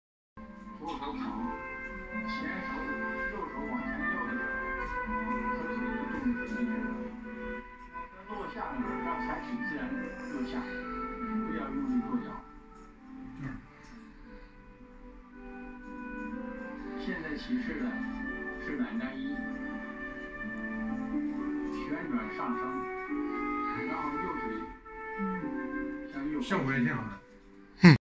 93 years old last living disciple of Chen Fa Ke, 10th gen. Chen style Taijiquan 2 sitting practice
March 2019,  the only one, last living disciple of grandmaster Chen Fa Ke陈发科 in age of 93, , master Tian Jianhua田剑华, talks essences of Chen style Taijiquan陈式太极拳. Master Tian, brother of more famous Tian Xiuchen(田秀臣) is really what we call "indoor" disciple, since Chen  Fa Ke taught him and other Beijing disciples just in their Tian family yard. His old concept of Qi and high level of the inner energy (Nei Gong内功) passed directly from Chen Fa Ke,  is very rare in late generations.  Wish long live to master Tian!